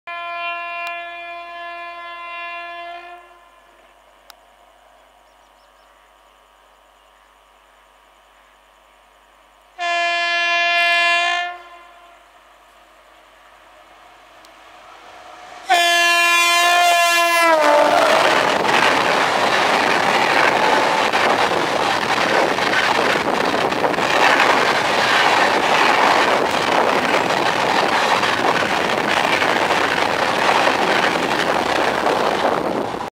200 Subscribers Special : Aggressive Chennai Duronto slashes at full pace !!
My channel reached a landmark of 200 subscribers and I thank each and everyone for the great support :)

Location -  Sukhisewaniyan (SUW), Madhya Pradesh (WCR).

Aggressive 12270 NZM-MAS Chennai Duronto breaks the silence by its sore throat honk and smashing @ 120 kmph hauled by Giant RPM WAP-7 #30378..

The train's speed is recently being upgraded to 130 kmph in Hazrat Nizamuddin-Lalitpur Stretch.
This is a bi-weekly train which connects Chennai to National Capital..